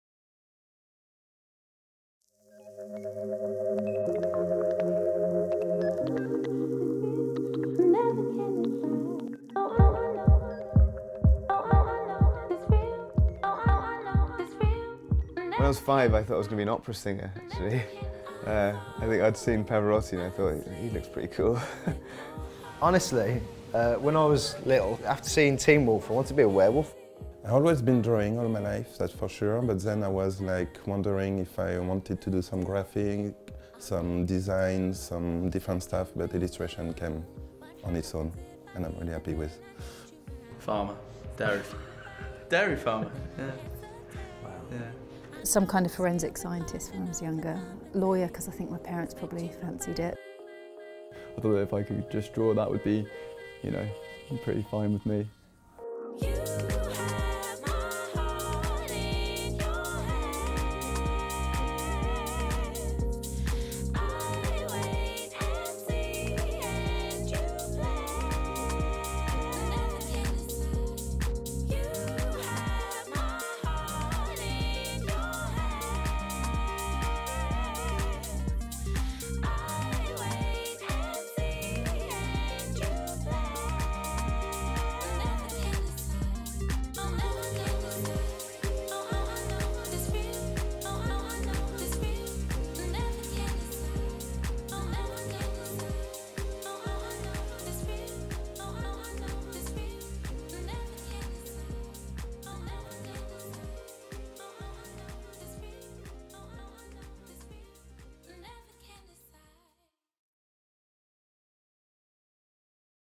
Pick Me Up 2014: Meet the Artists
Now in its fifth year, the UK's original contemporary graphic arts festival Pick Me Up showcases graphic art in all its forms. Aimed at being the antithesis of a traditional art fair, Pick Me Up is a fun and informal festival featuring quirky studio spaces in which to shop for affordable artwork from the great and good of graphic arts, and a daily, lively line-up of events which encourage both budding and bonafide artists of all ages to get involved.

Produced by PAL

Director: Angus and Eoin
DOP: Joe Mannion
Animation:Zach Ellams
Editing: Angus Dunsire

Andrew Groves, Brothers of the Stripe, Handsome Frank, Edward Carvalho-Monaghan, Thibaud Herem, Lynnie Zulu, Andrew Martin, Puck Studios, Animaux Circus, Jo Ratcliffe, Annu Kill.

Special Thanks to Chocodeli, Pixi Pixel, Noah Ashford, Brownswood Recordings and Anushka